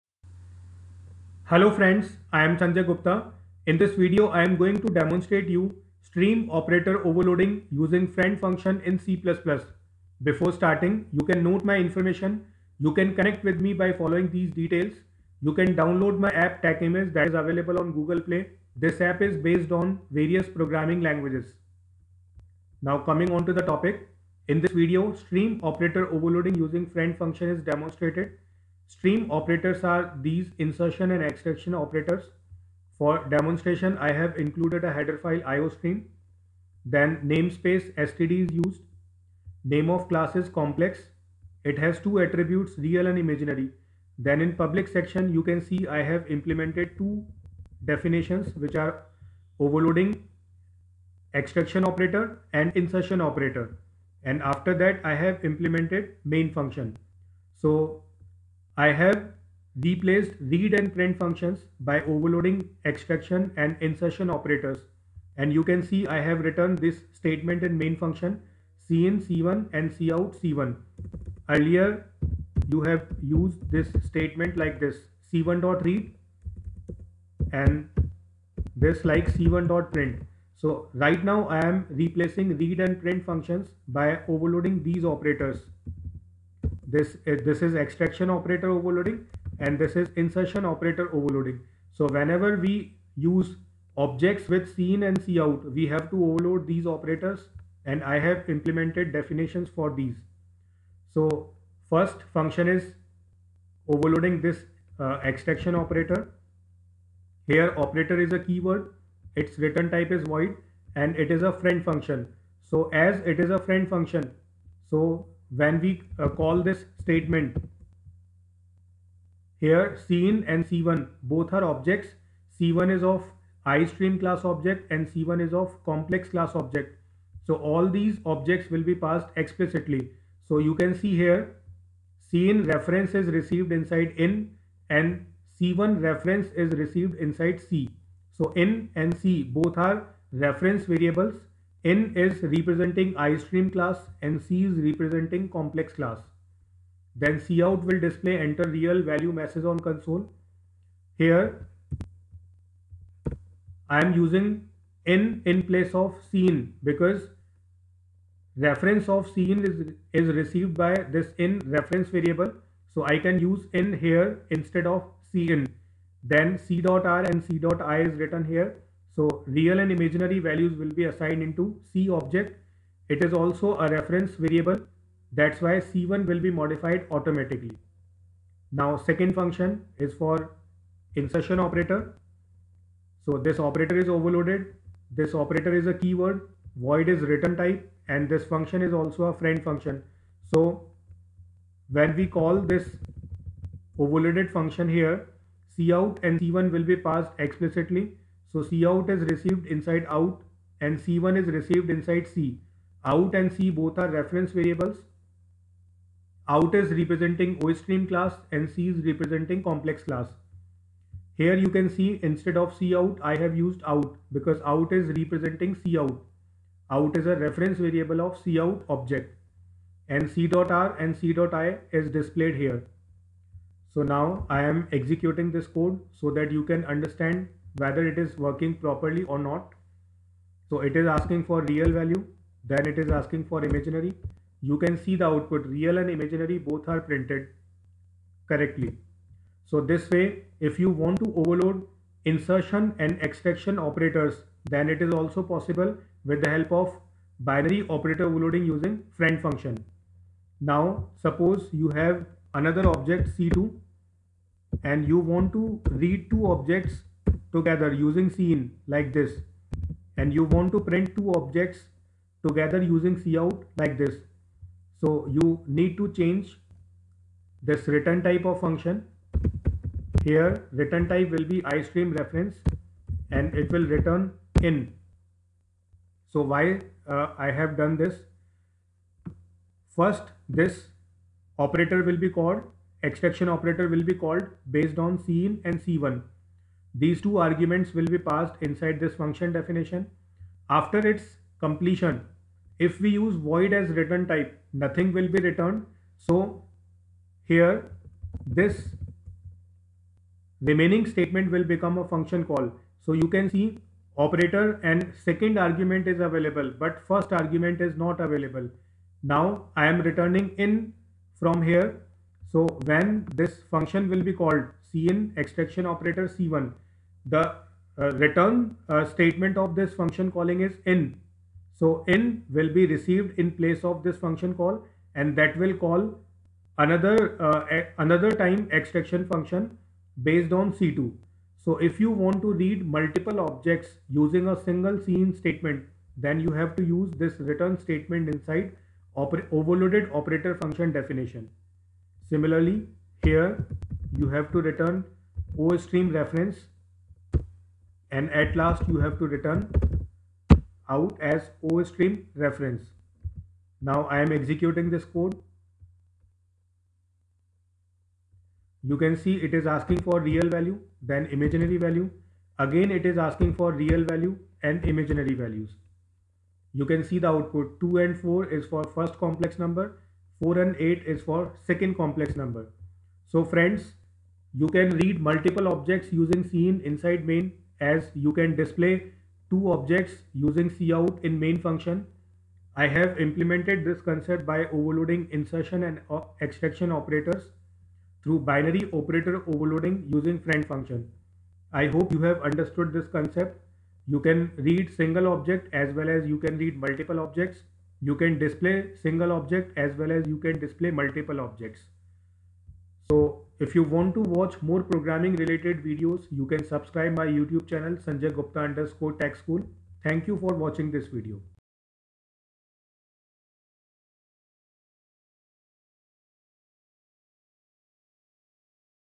61 C++ | How to implement stream operator overloading using friend function in C++
Program to implement stream operator overloading using friend function in C++ programming language.